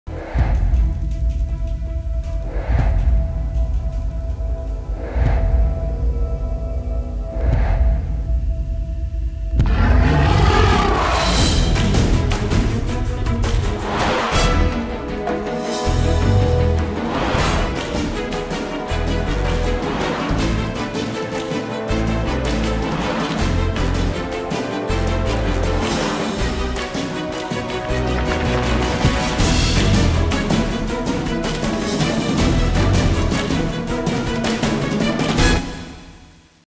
Dinosaur Park Sizzle
Dinosaur Park - Promotional video for new book from Kingfisher Publications coming in October 2009.